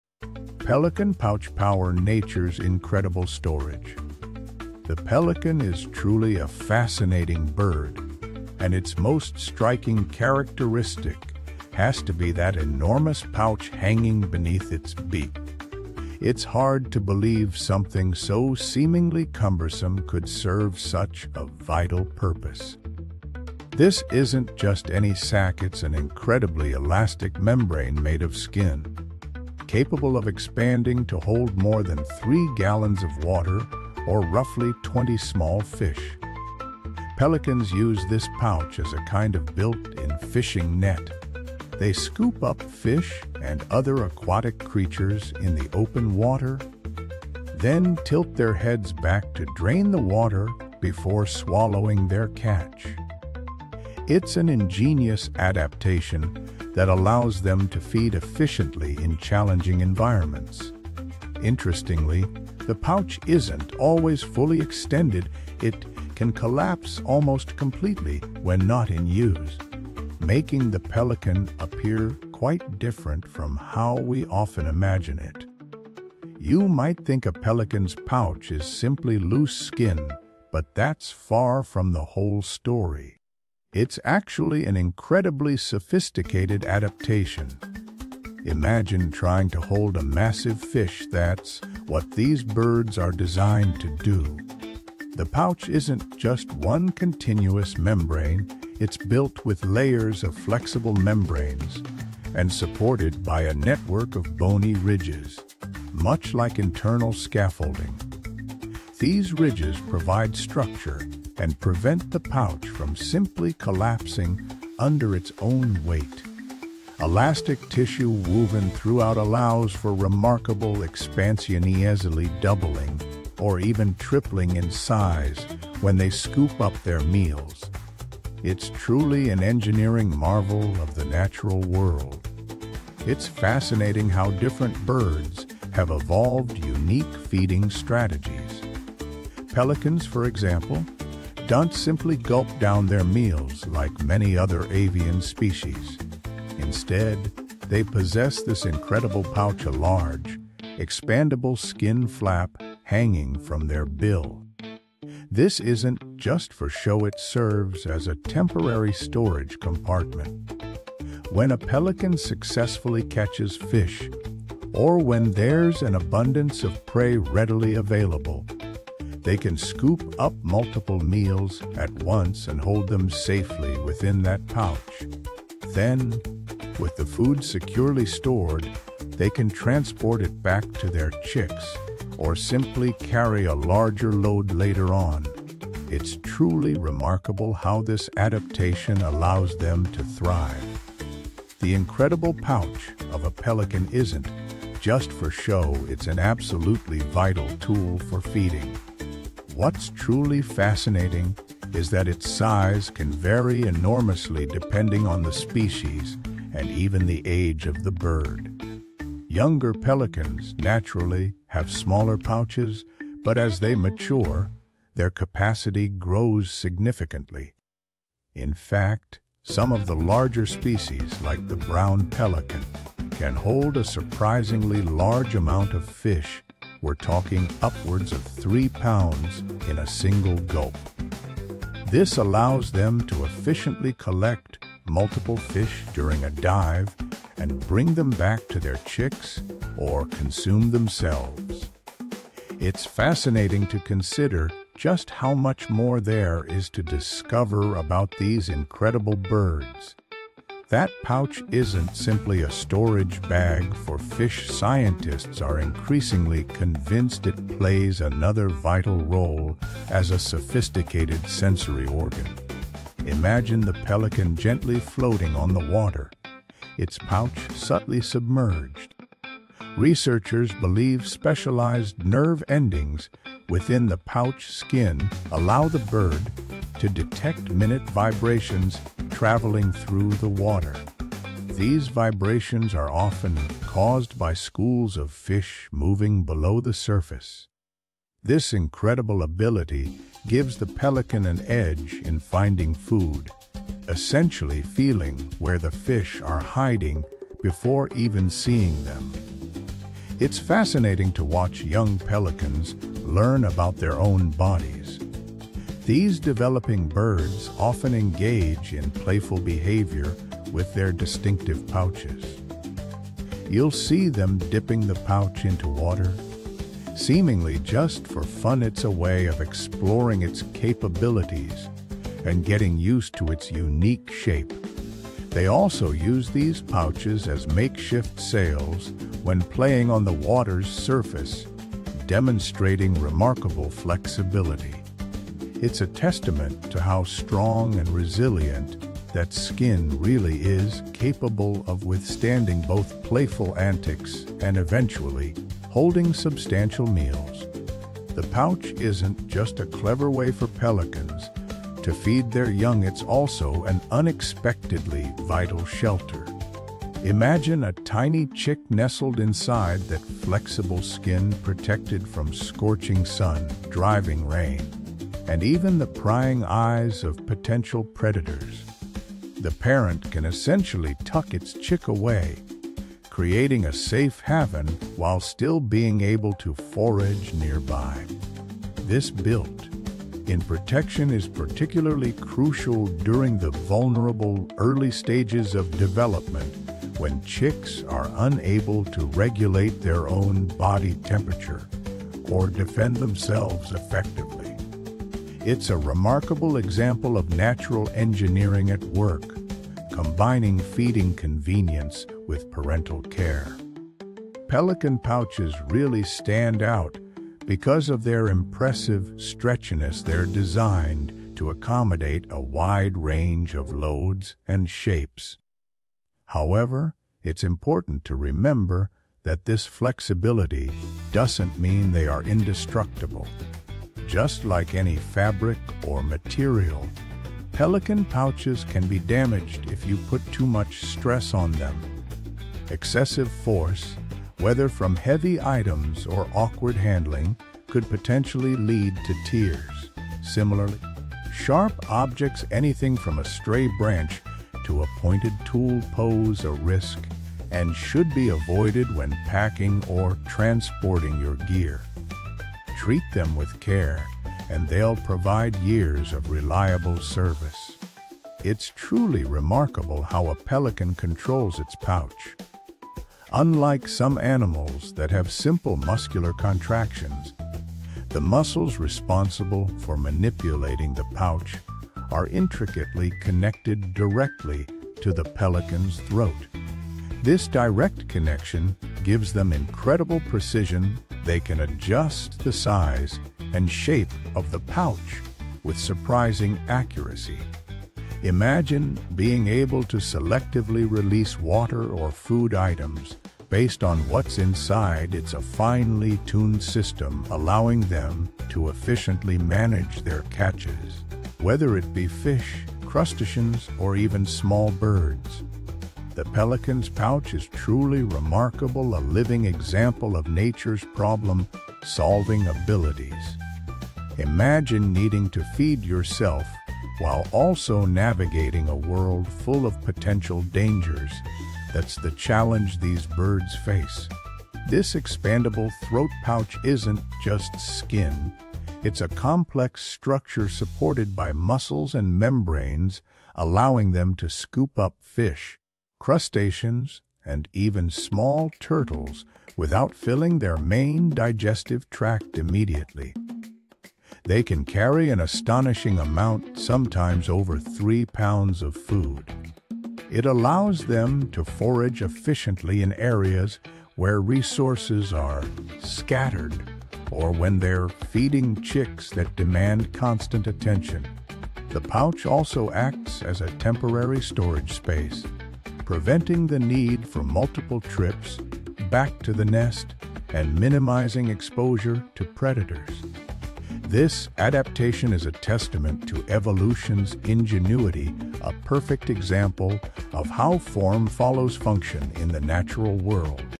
Pelican Pouch Power: Nature's Incredible Storage!
Welcome to an exploration of one of nature's most astonishing adaptations: the Pelican's pouch! This video dives deep into the fascinating world of these coastal birds, focusing specifically on their incredible throat pouches and how they utilize them. We’ll uncover the secrets behind this unique feature, from its structure and function to its role in raising young and even playing games.